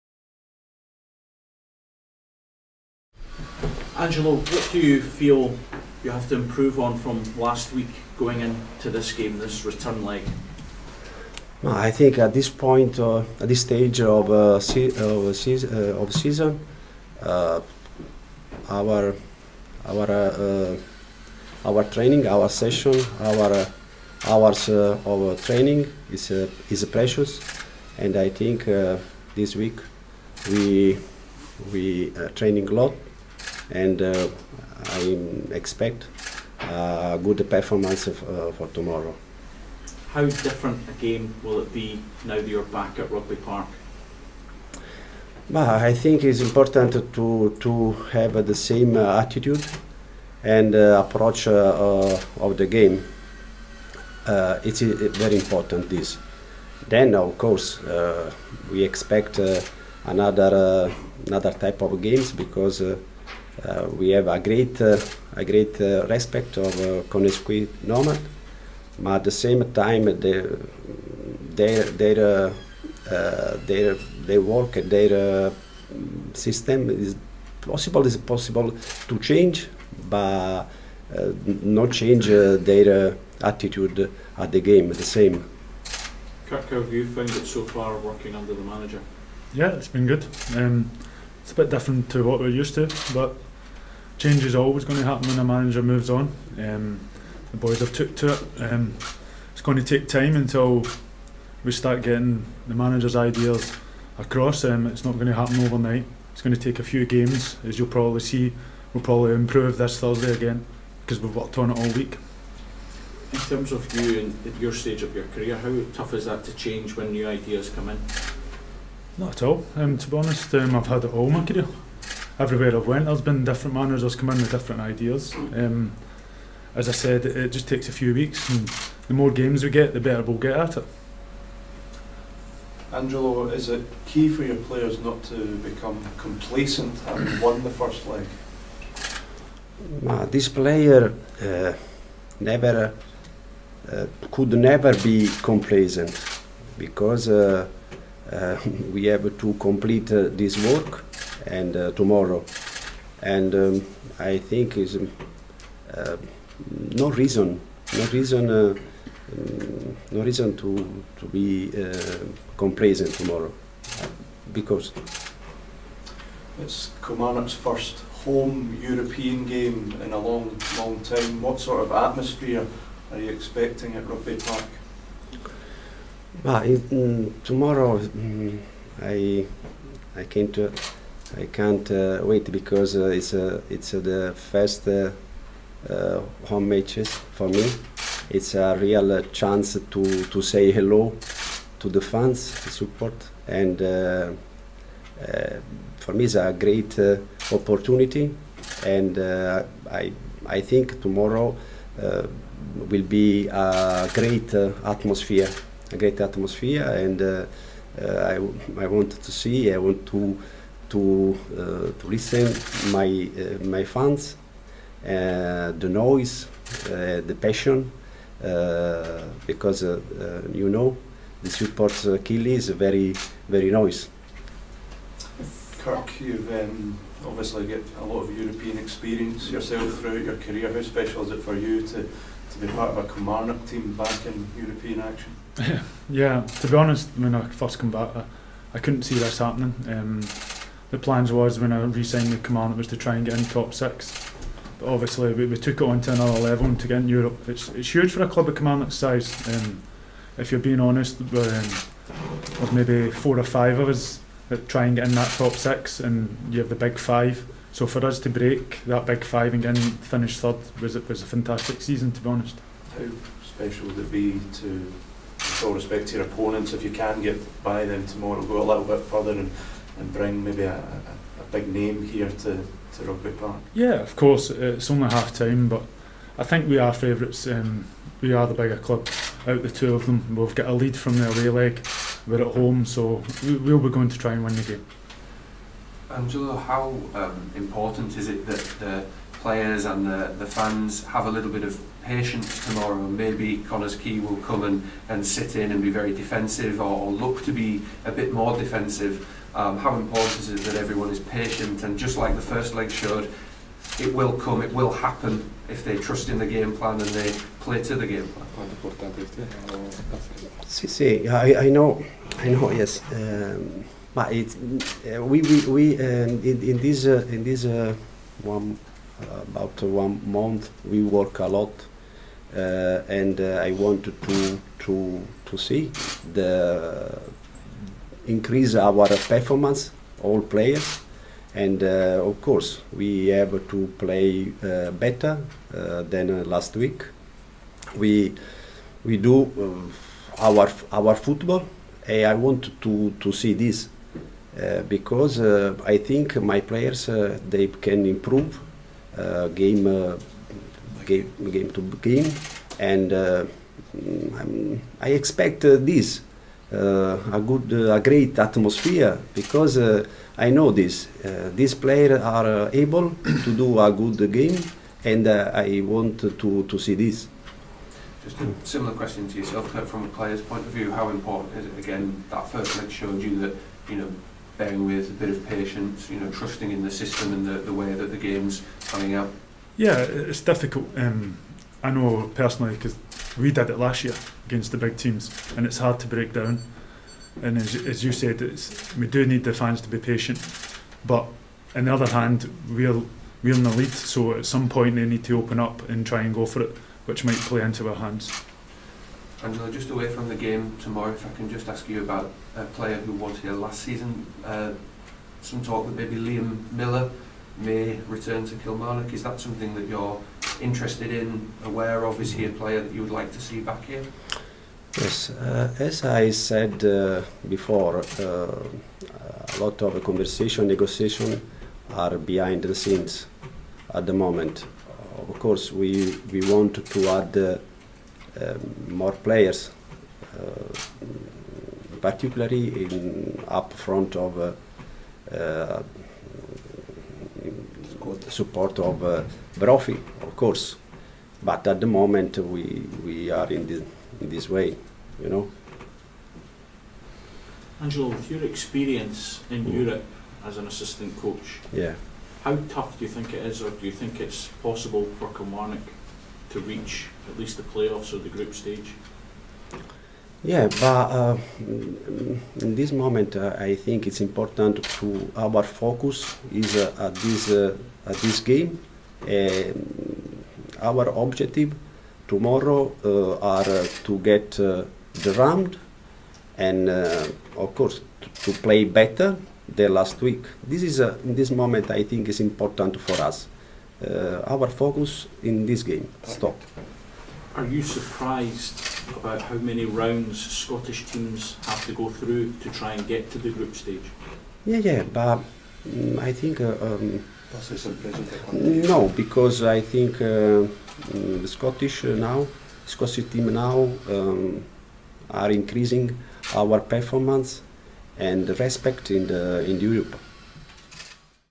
Angelo Alessio and Kirk Broadfoot | Media Call
Kirk Broadfoot joined the manager to look ahead to the boss' home debut.